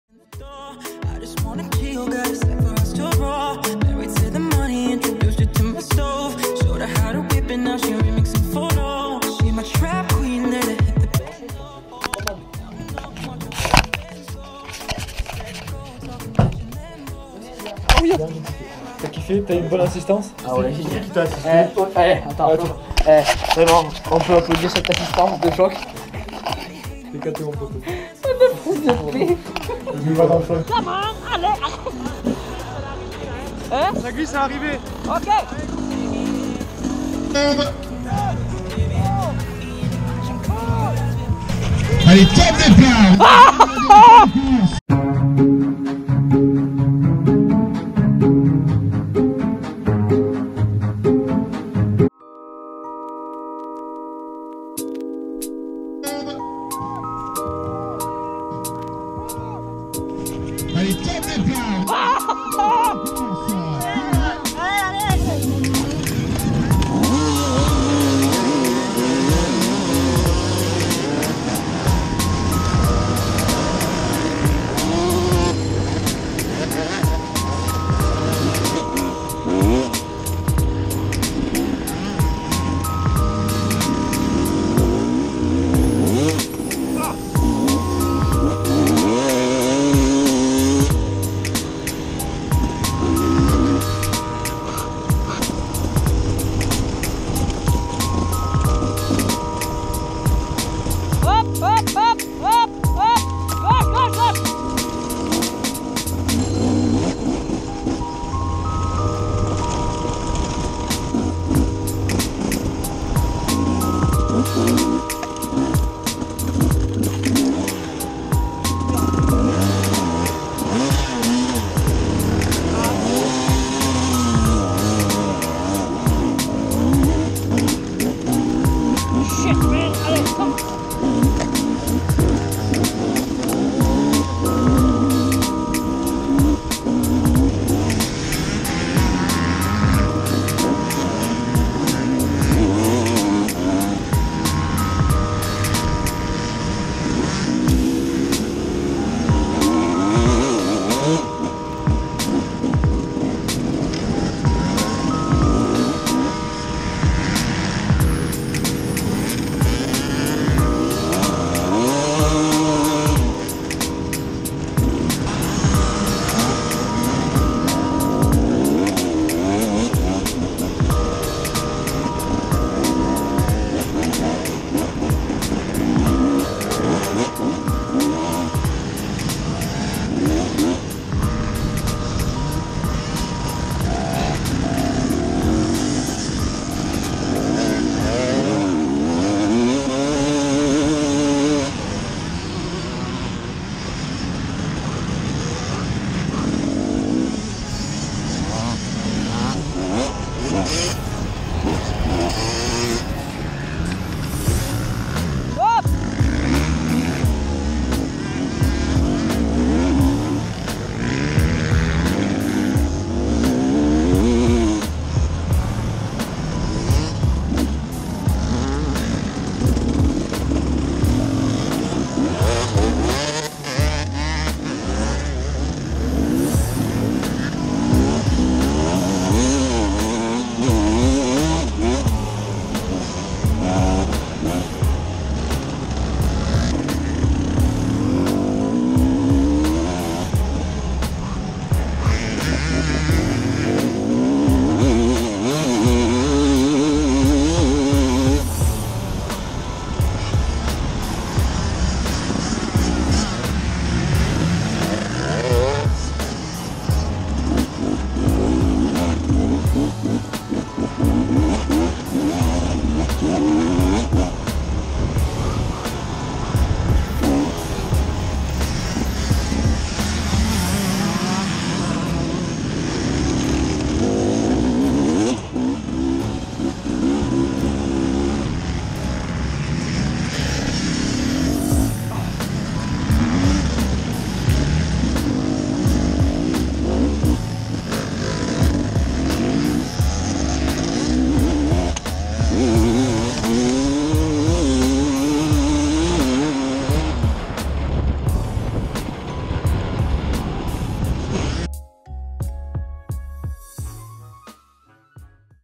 Endurance langeac 2016
petit retour sur l'endurance de langeac j'espere sa vous plairas j'ai pas pu filmer tt le temps car j'a eu des probleme avec ma go pro et aussi car  ell me gener..
Bon visionnage ;)

Mon snap: dansagara13